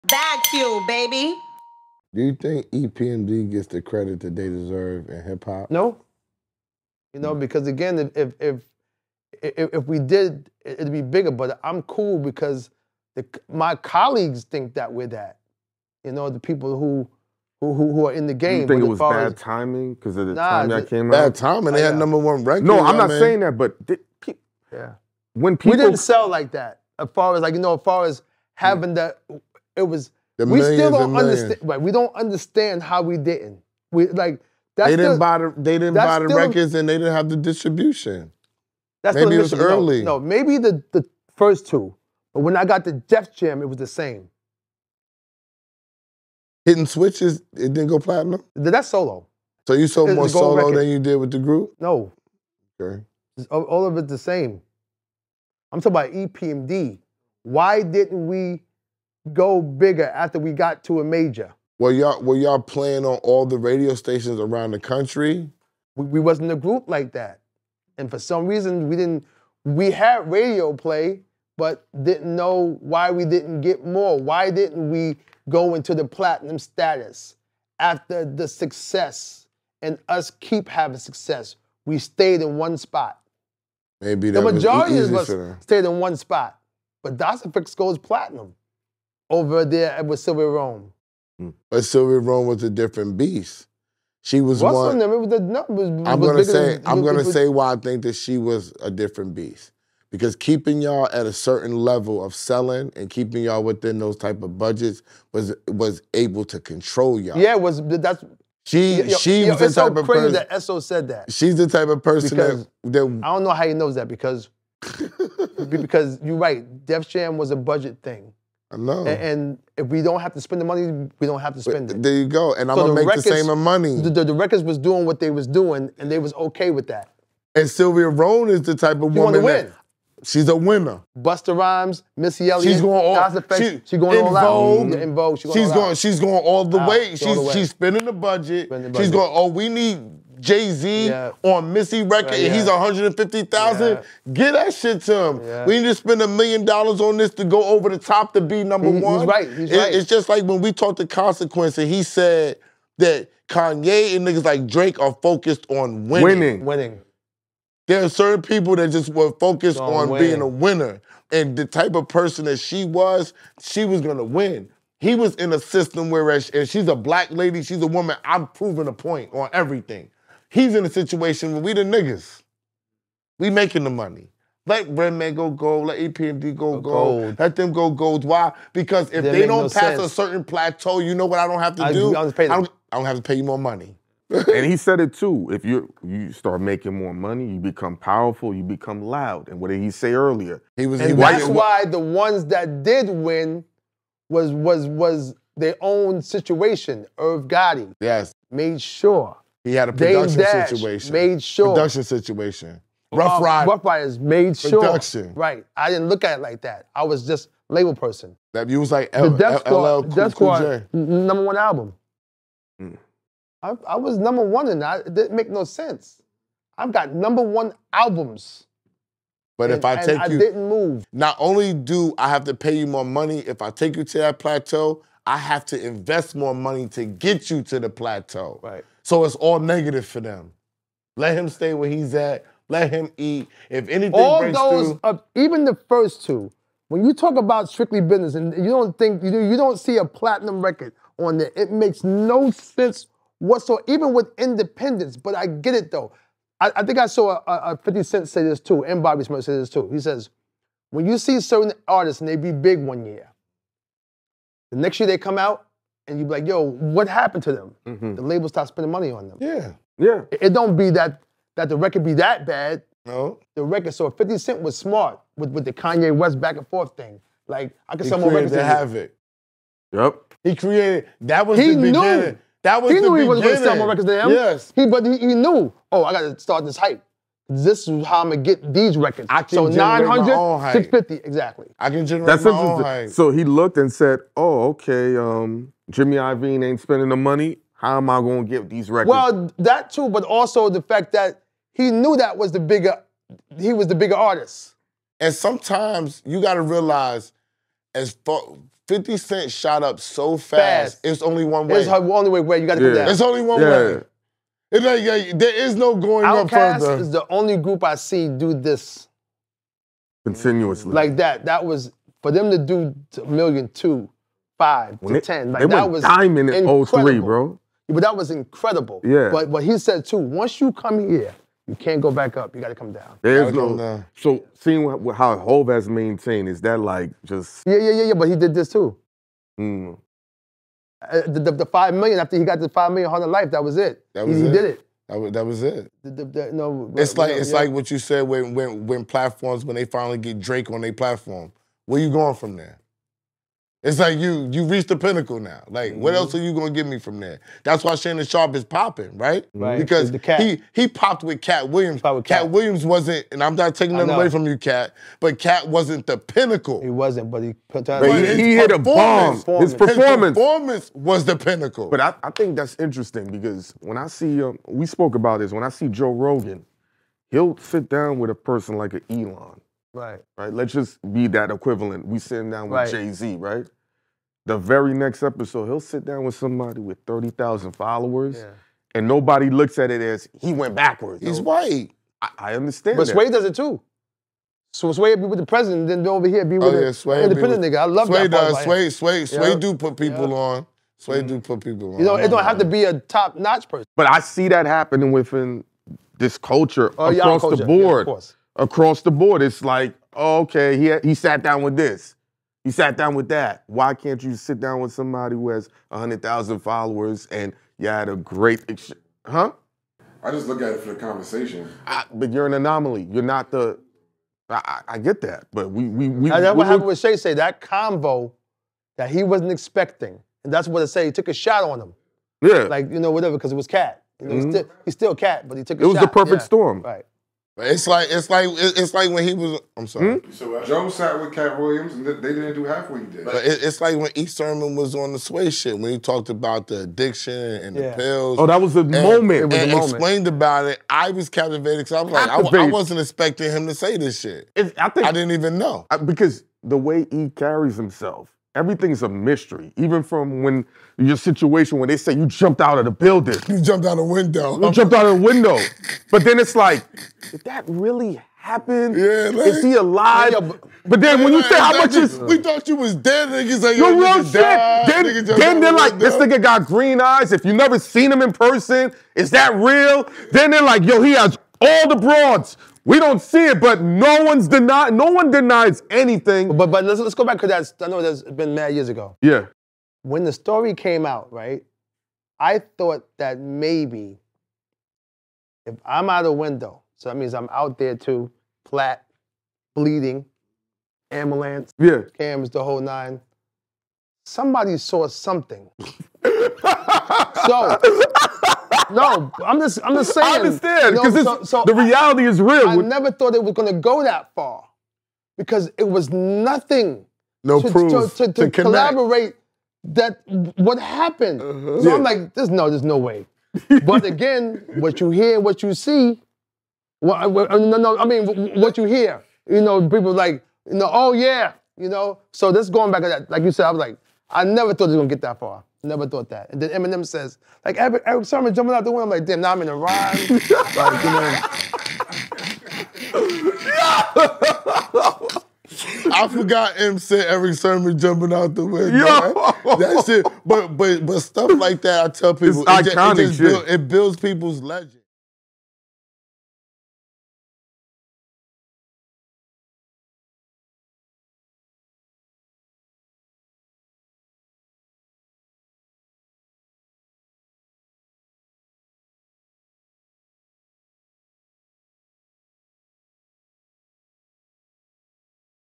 “They Stopped Spending The MONEY!!!” | Erick Sermon On Why (EPMD) Never Went Platinum 💿
Erick Sermon uncovers the untold story behind EPMD's lack of recognition in the music industry.